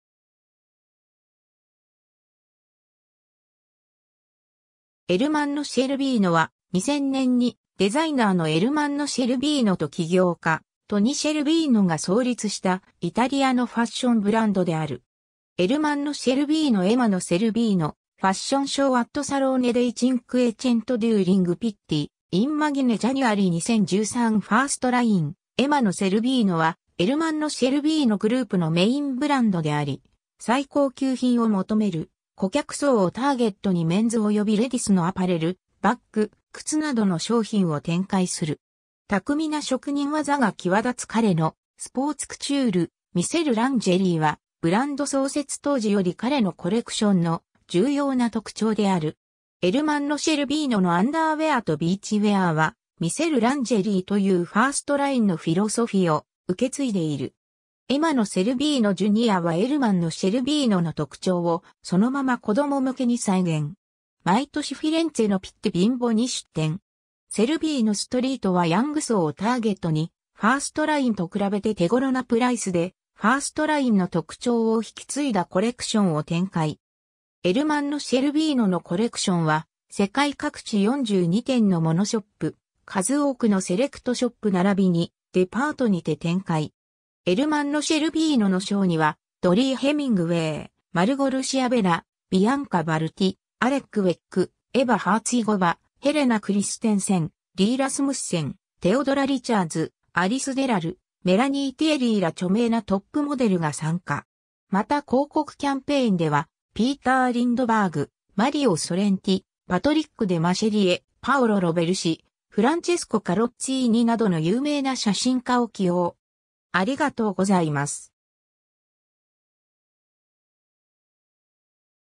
エルマンノ・シェルヴィーノ
エルマンノ・シェルヴィーノ（伊: Ermanno Scervino）は、2000年にデザイナーのエルマンノ・シェルヴィーノと企業家トニ・シェルヴィーノが創立したイタリアのファッションブランドである。
 エルマンノ・シェルヴィーノ（2011年） Ermanno Scervino fashion show at Salone dei Cinquecento during Pitti Immagine January 2013 ファーストライン「Ermanno Scervino」はエルマンノ・シェルヴィーノ・グループのメインブランドであり、最高級品を求める顧客層をターゲットにメンズおよびレディスのアパレル（プレタポルテ）、バッグ、靴などの商品を展開する。
巧みな職人技が際立つ彼の「スポーツクチュール」、「見せるランジェリー」は、ブランド創設当時より彼のコレクションの重要な特徴である。
 エルマンノ・シェルヴィーノのアンダーウェアとビーチウェアーは、“見せるランジェリー”というファーストラインのフィロソフィを受け継いでいる。
 「Ermanno Scervino Junior」はエルマンノ シェルヴィーノの特徴をそのまま子供向けに再現。
毎年フィレンツェのピッティ ビンボに出展。
 「Scervino Street」はヤング層をターゲットに、ファーストラインと比べて手ごろなプライスでファーストラインの特徴を引き継いだコレクションを展開。
 エルマンノ・シェルヴィーノのコレクションは、世界各地42店のモノショップ、数多くのセレクトショップならびにデパートにて展開。
 エルマンノ・シェルヴィーノのショーには、ドリー・ヘミングウェイ（Dree Hemingway）、マルゴルシア・ベラ（Malgosia Bela）、ビアンカ・バルティ（Bianca Balti）、アレック・ウェック（Alek Wek）、エヴァ・ハーツィゴヴァ（Eva Herzigova）、ヘレナ・クリステンセン（Helena Christensen）、リー・ラスムッセン（Rie Rasmussen）、テオドラ・リチャーズ（Theodora Richards）、アリス・デラル（Alice Dellal）、メラニー・ティエリー（Melanie Thierry）ら著名なトップモデルが参加。
また広告キャンペーンではピーター・リンドバーグ（Peter Lindbergh）、マリオ・ソレンティ（Mario Sorrenti）、パトリック・デマシェリエ（Patrick Demarchelier）、パオロ・ロヴェルシ（Paolo Roversi）、フランチェスコ・カロッツィーニ（Francesco Carozzini）などの有名な写真家を起用。